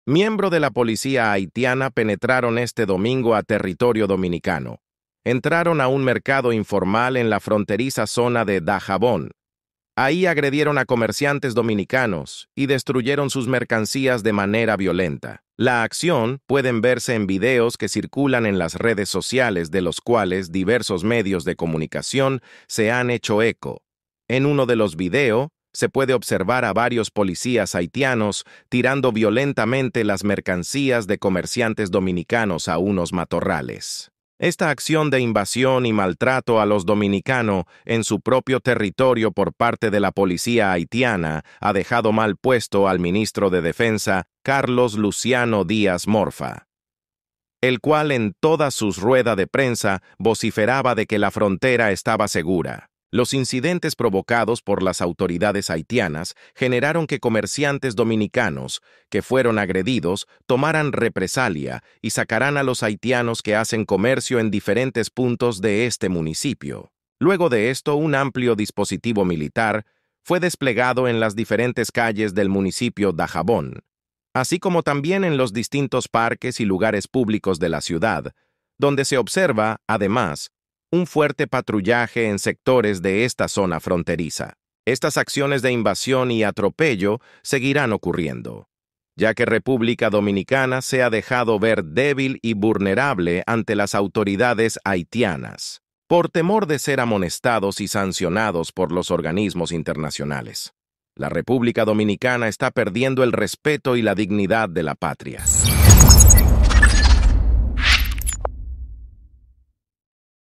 PARECIERA QUE DOMINICANA LE TIENE MIEDO A HAITÍ 🤔
Militares haitianos cruzaron la frontera del lado dominicano este domingo donde maltrataron comerciantes dominicanos y destruyeron sus mercancías

para 👉 cooperaciónes, promociones colaboraciones, y publicidad comercial escríbenos a